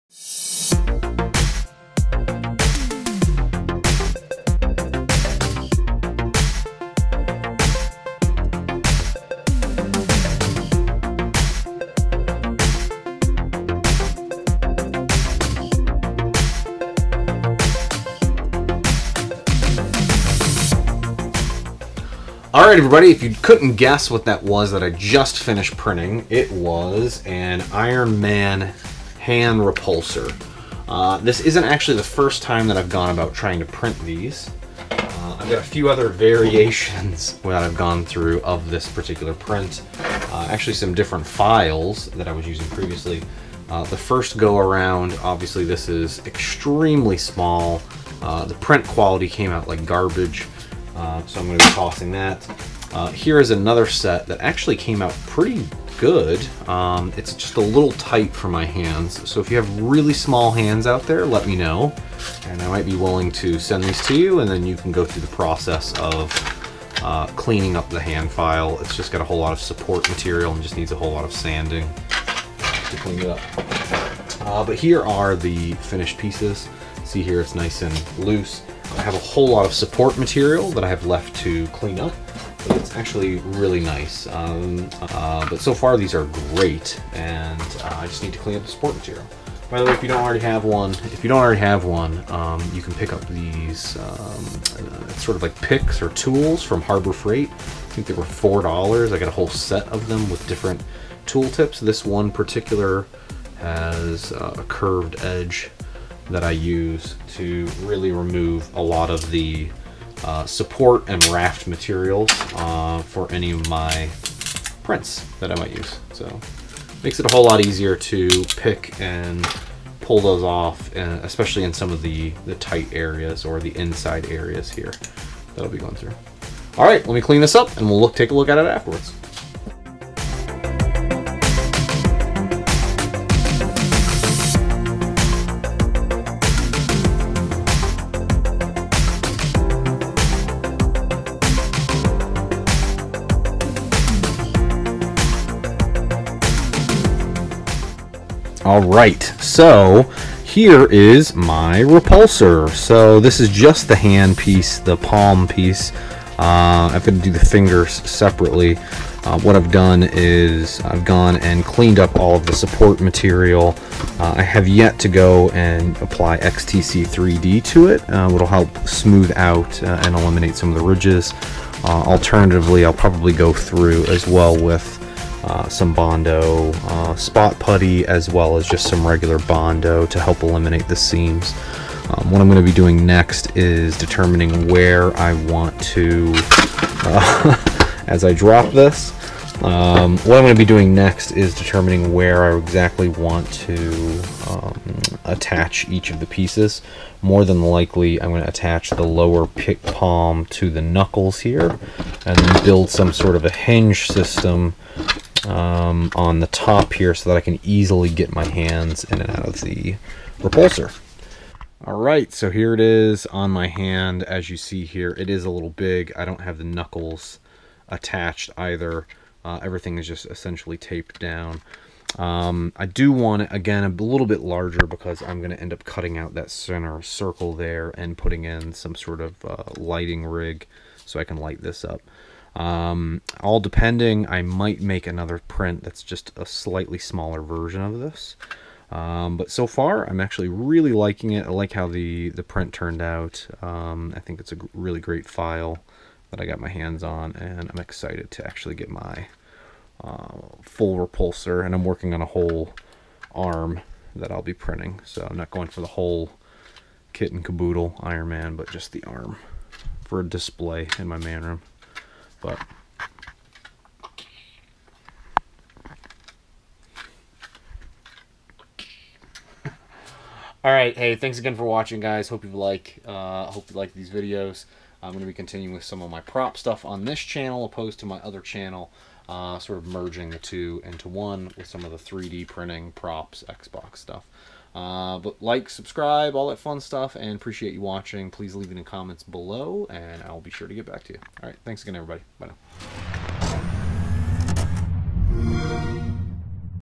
3D Printed Iron Man Repulsor
Checkout my latest project... IRON MAN Gauntlet!!!  This is probably the 6th time I've started this project... hopefully I can finally see it through to the end.